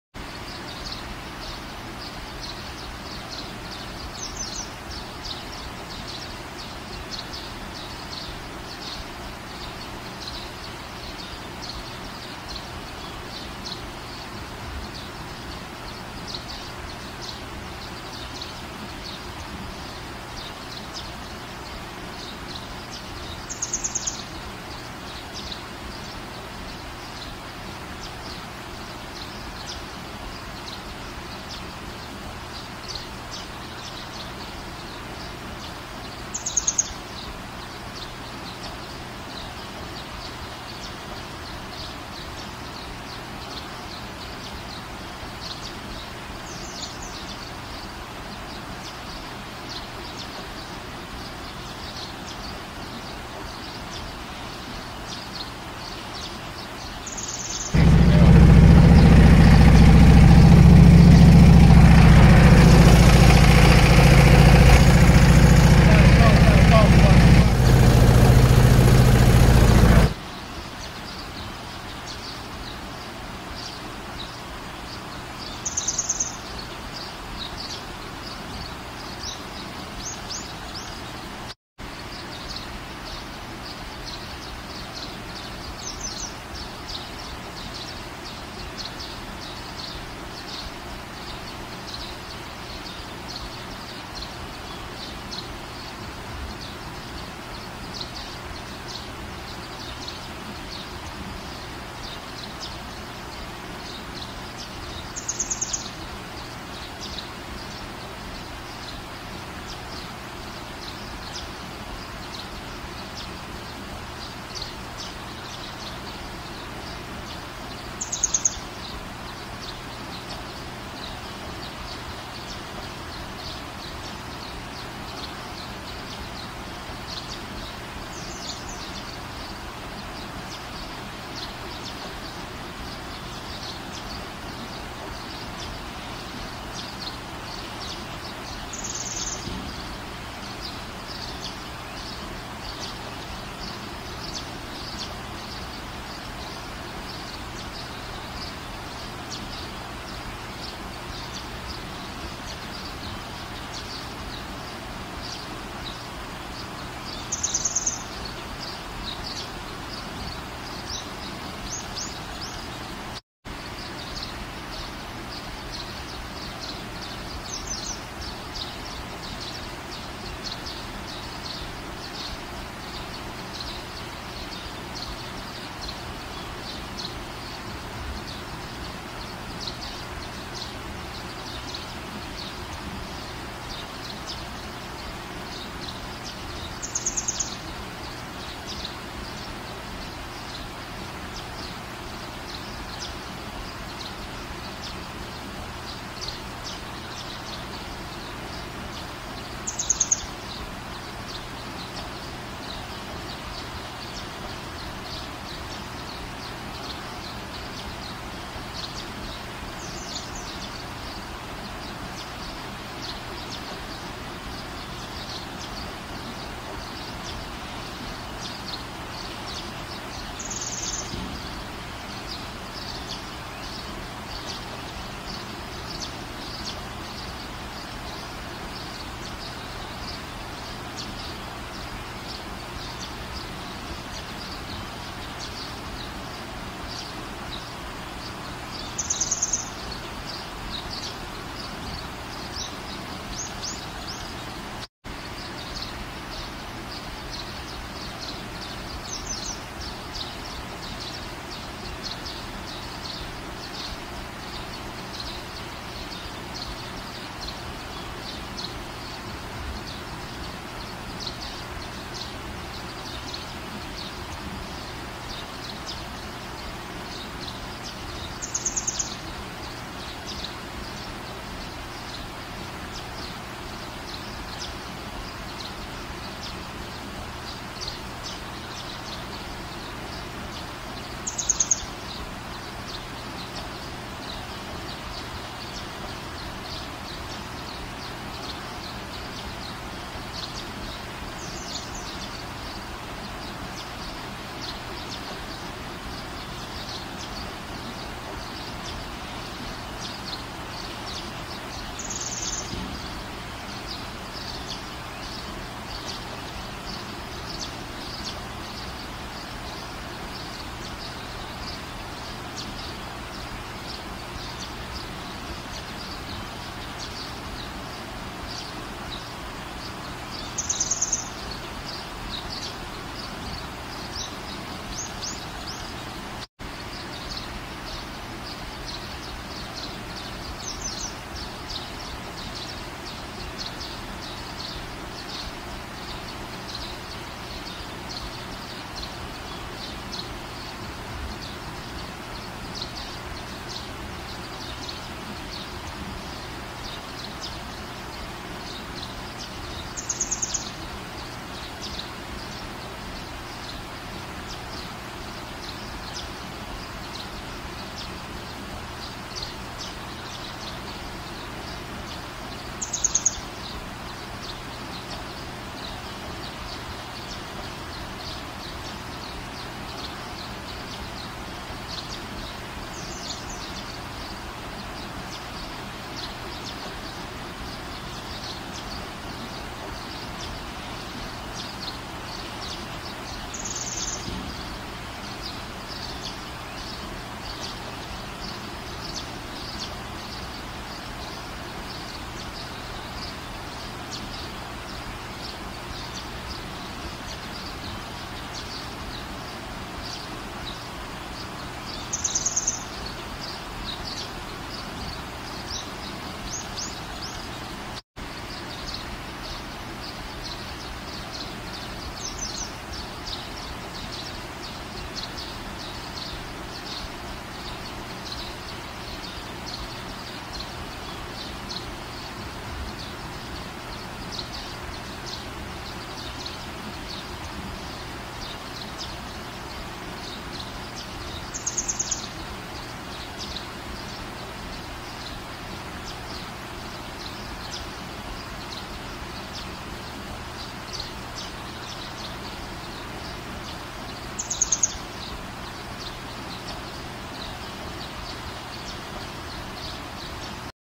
Expensive Datina Fish Maw How To Drying Expert | How To Drying Ghol Fish Maw
Datina fish maw is not easy to dry. Because it is not a cheap product like other fish maw. It is very expensive fish maw.  Due to which it has to be very careful while drying. Those who have been running the fish maw business for a long time and can keep the quality good must dry the